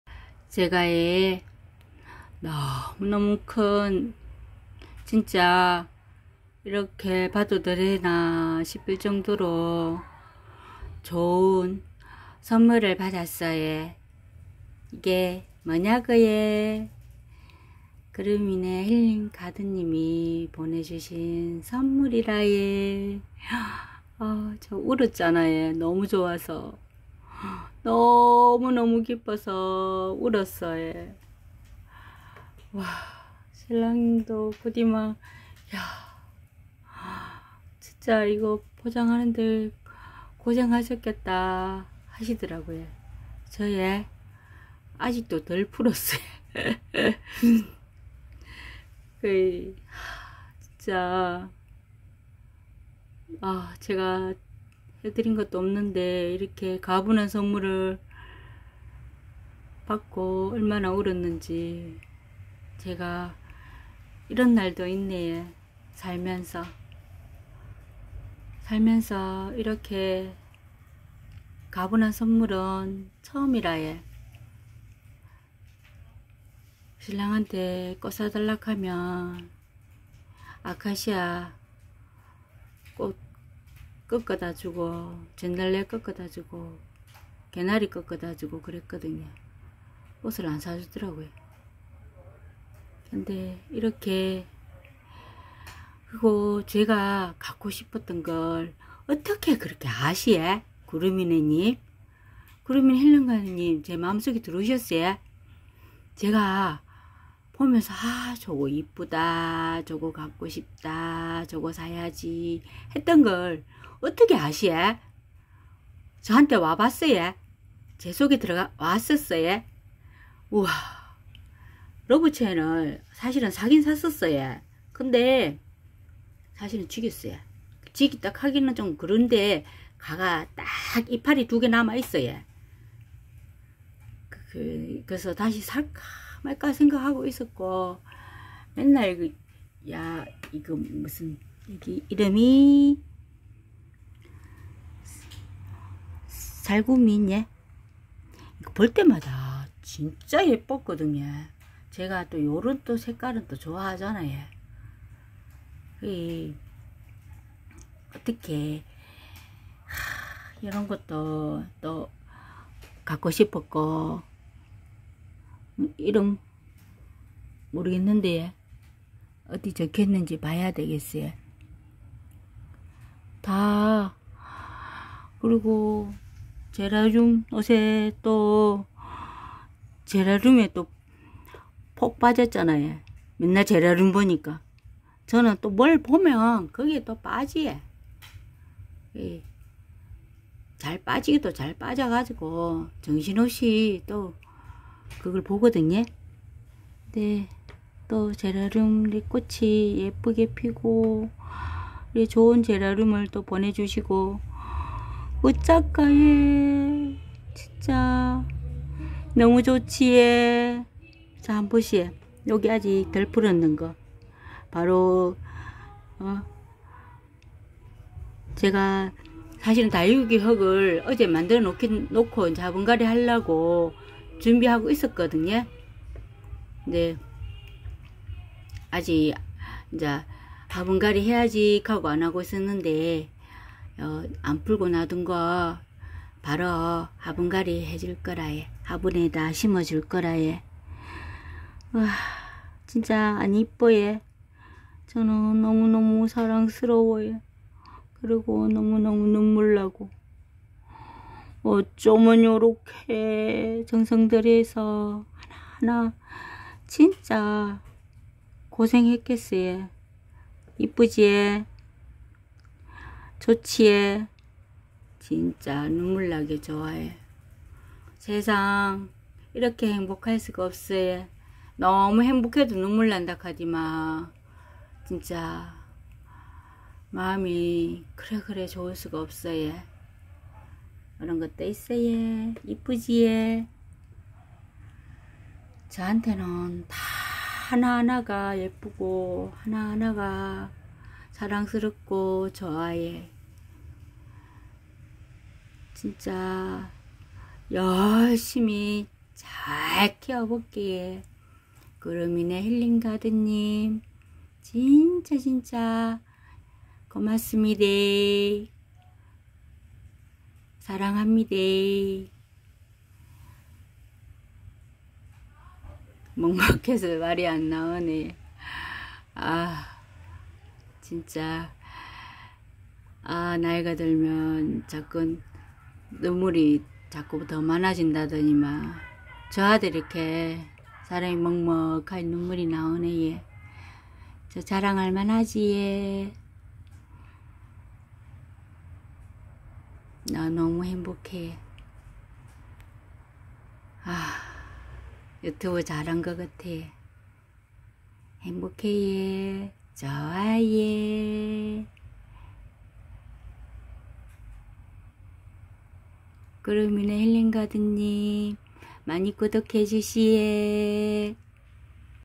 나를 울린 유튜브 구름이네 힐링가든님[과분한 선물 받았어요 자랑해요. 구름이네 힐링가든님 채널 구독하셔요 많은 정보 알려 주셔요 많으걸 배워요
구름이네힐링가든 유튜브초보 같은 영상을 올리니. 구름이네 힐링가든님께서 크리에이티브 커먼즈를 하라해서 유튜브 열심히 검색해서 시간여행하는 영상들 변경했어요 초보 유튜브인 저에게 이것저것 알뜰살뜰 알려주시는 구름이네 힐링가든님 채널 구독하셔요